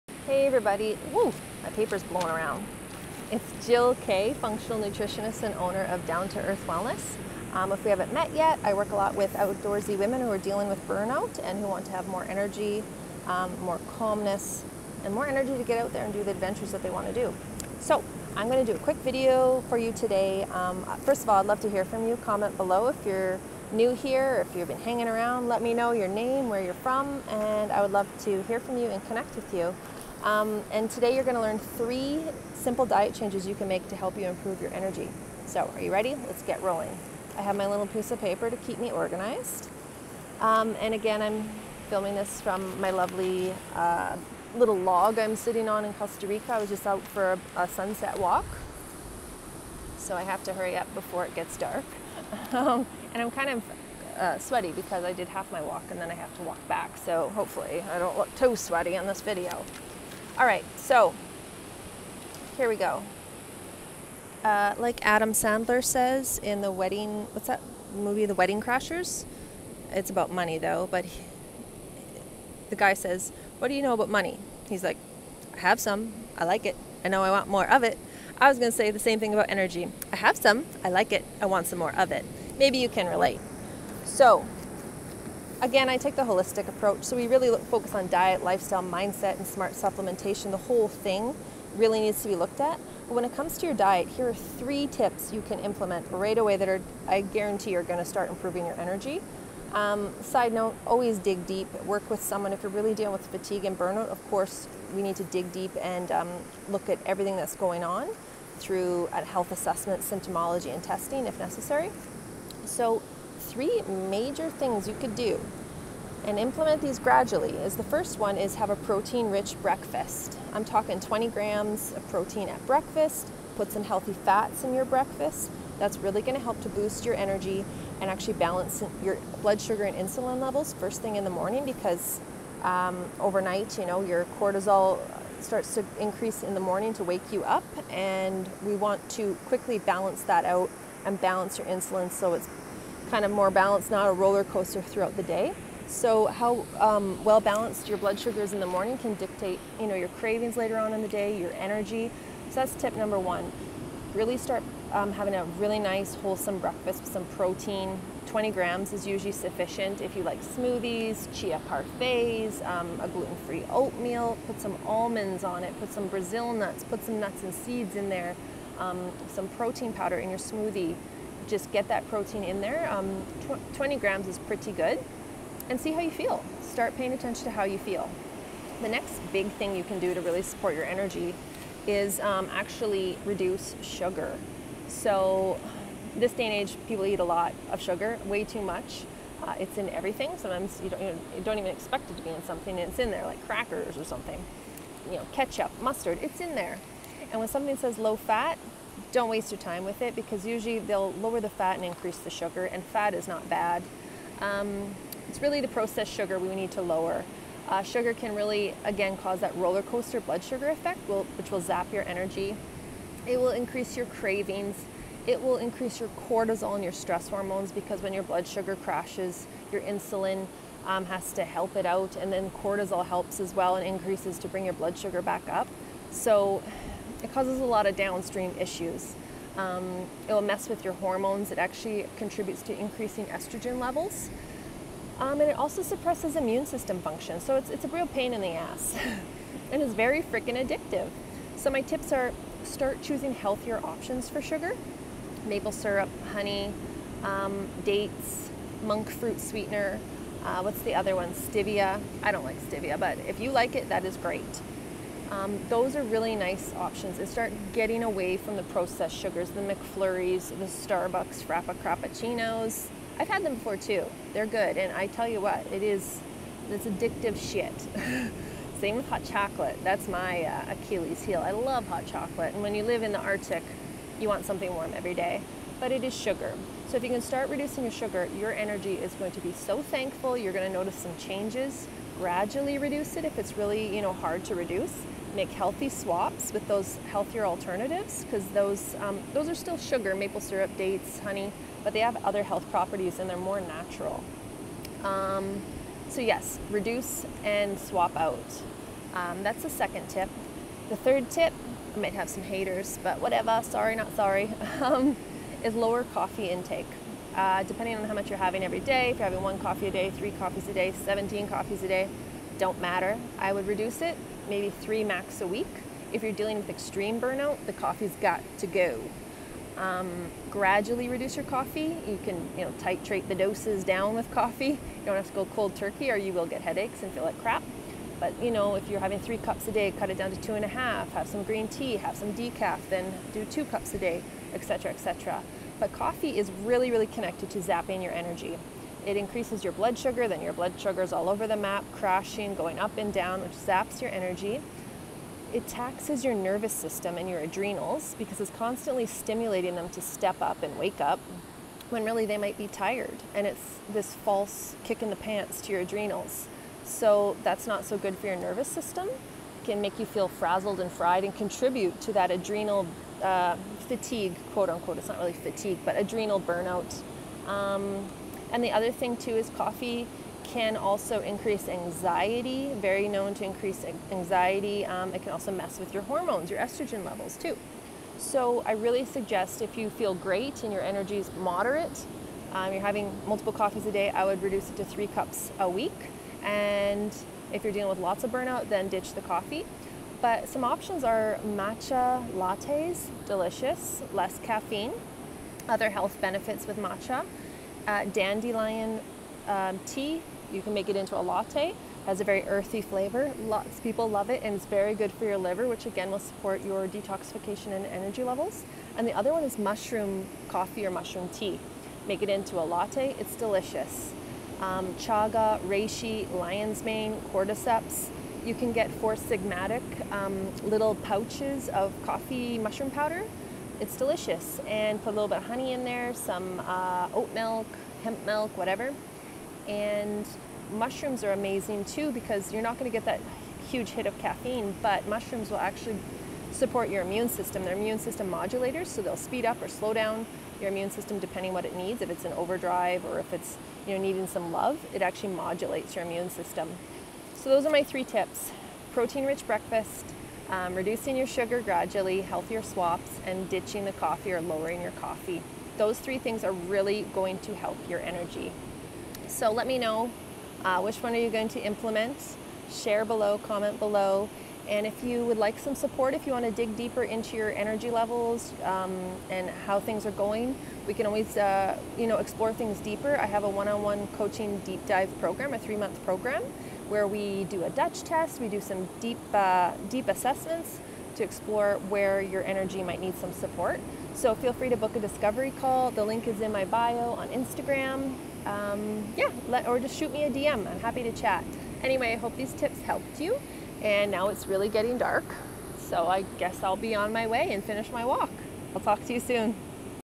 3 quick diet changes you can make now for more energy
Want more energy but not sure where to start? In today's quick video, I share 3 quick diet changes and upgrades that you can make pronto in order to have more energy and feel your best. No quick fixes, no diet gimmicks, just smart optimization of your diet to help you see results. Hope you enjoy today's video!